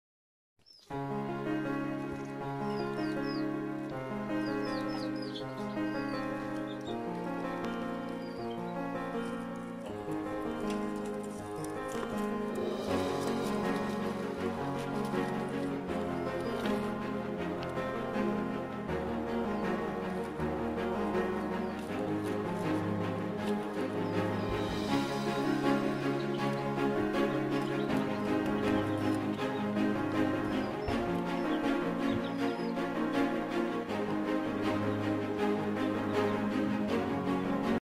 Bébés lapins angora nains 3 jours
Voici une vidéo des bébés de Youpi et Marley. Sur cette vidéo ils ont 3 jours.